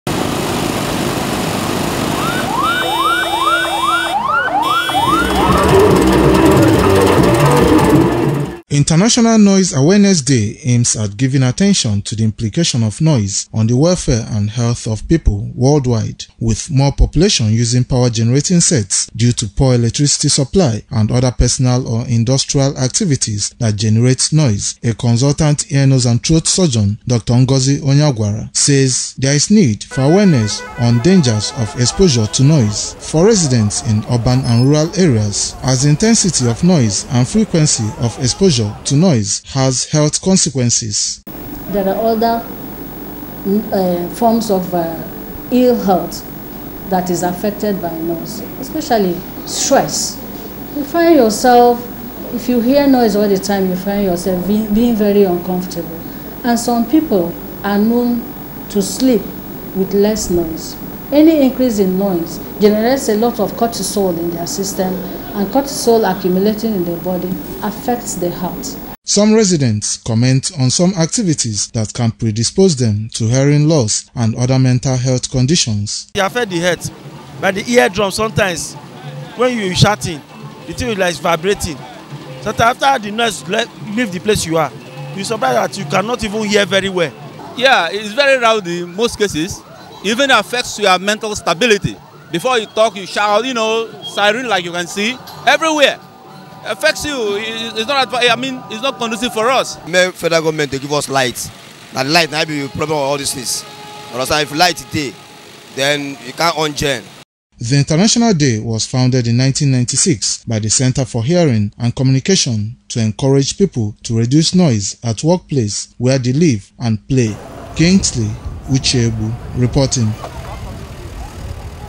Frequent exposure to noise attributed to some risk factors
Frequent exposure to power generating sets and other human activities has been attributed to some risk factors associated with hearing loss, stress and cardio vascular diseases.

Kingsley Uchegbu spoke with a medical expert on this year’s International Noise Awareness Day.